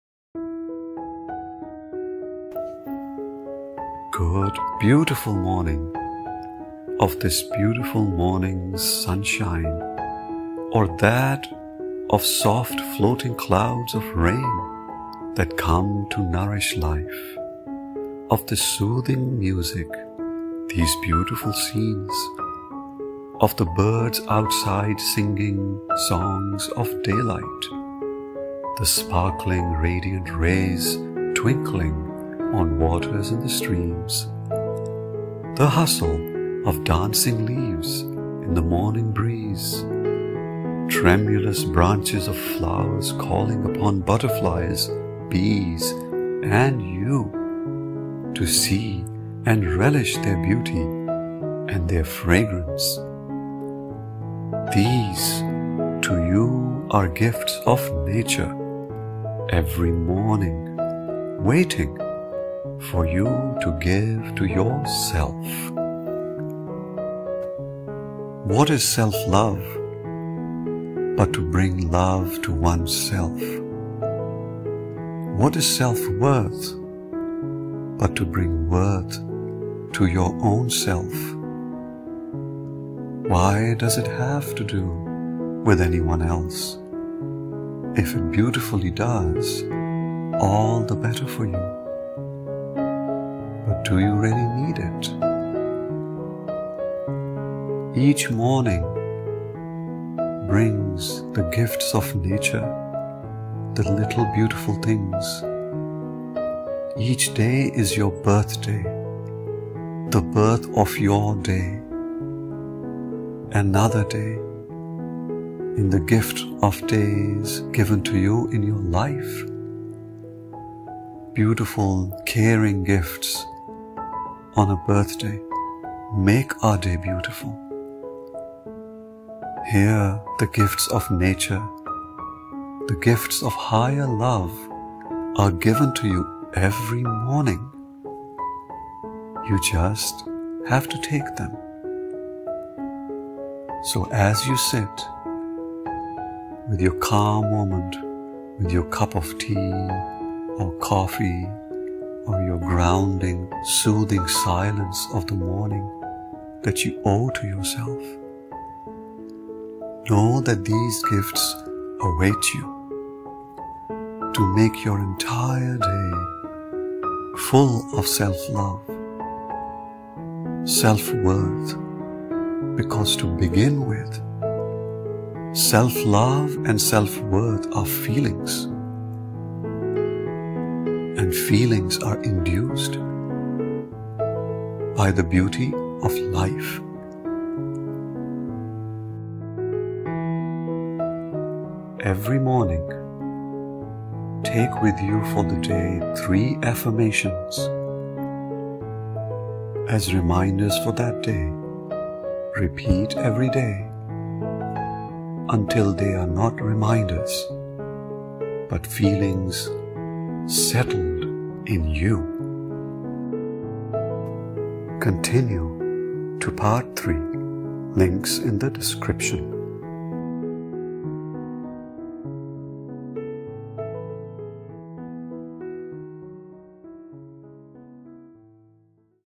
Self-Love's Exquisite Morning Affirmations - P 2/3 - Blissfully Musical & Scenic Morning Meditation
Self-Love Series - Part 2: A Meditational & Blissful Scenic Morning Affirmations Set In Music.

Soothing and meditation like relaxing time for your mornings with affirmations for self-love and self worth set in three parts as a 10 minute morning meditation of affirmations with beautiful scenes and soothing music - to be played in series.

Wisdom Dive Channel